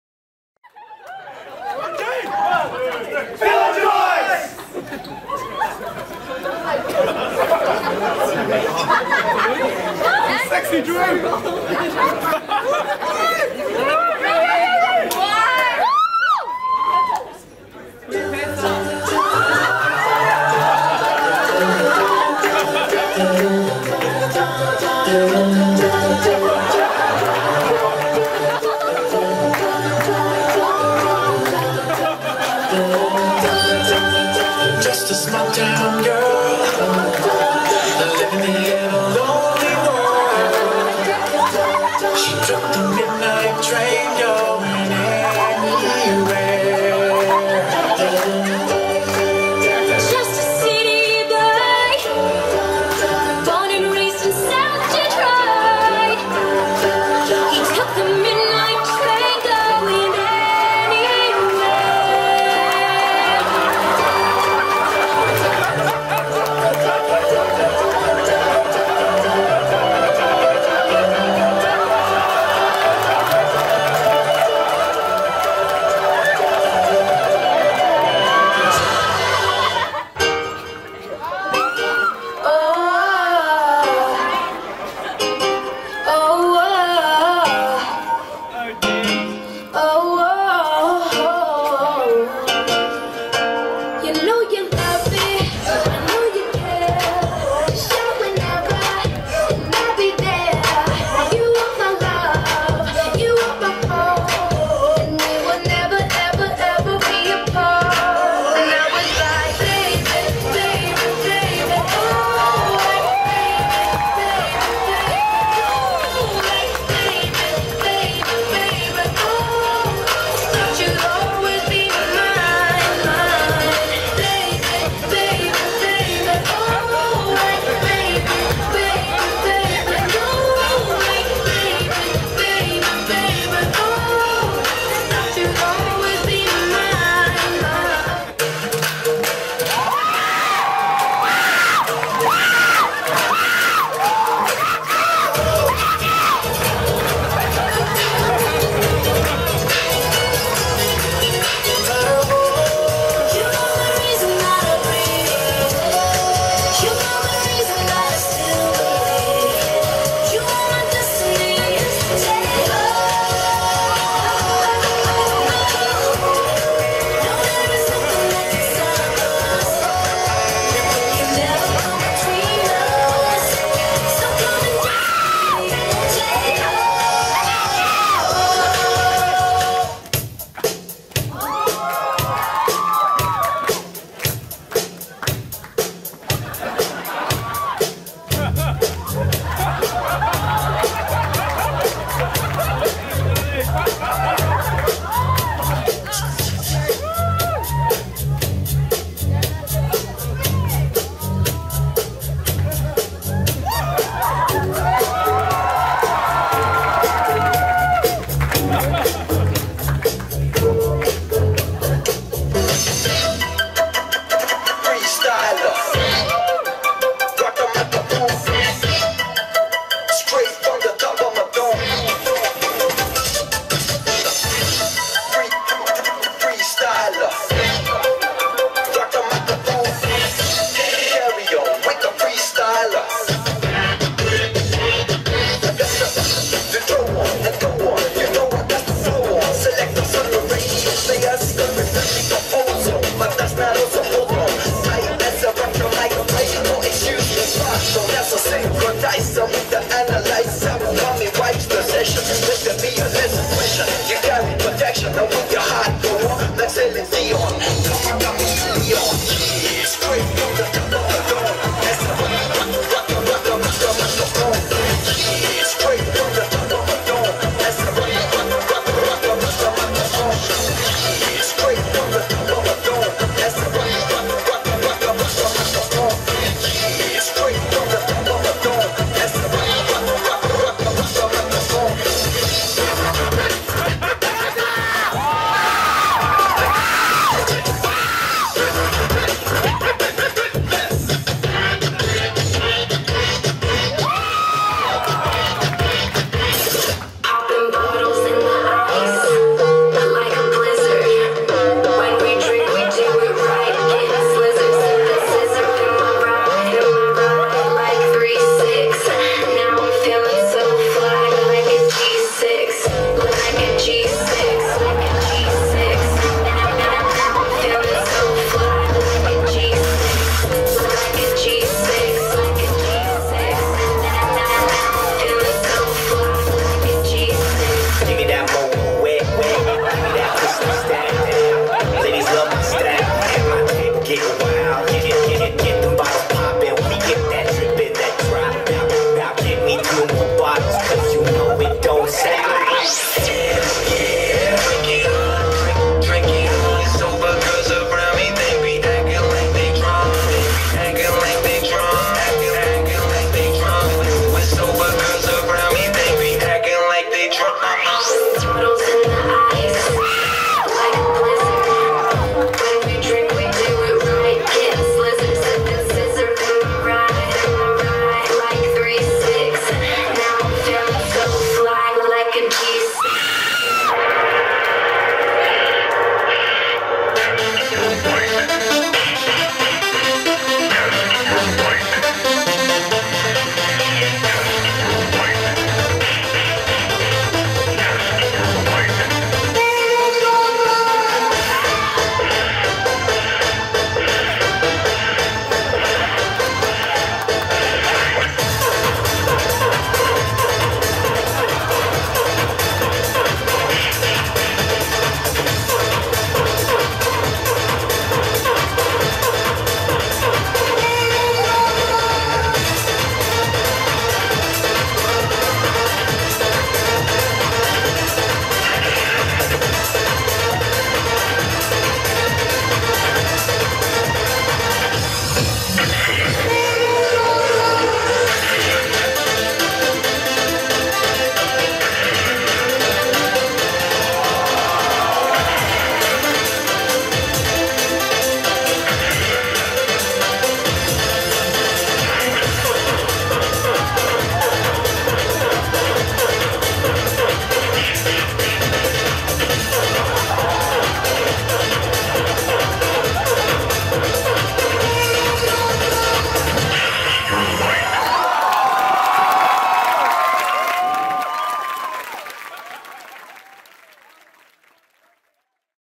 Wedding Dance: Phil & Joyce (2010)
Phil & Joyce's Wedding.
First dance item performed back in the days;
Previously corrupted somehow, so re-uploaded.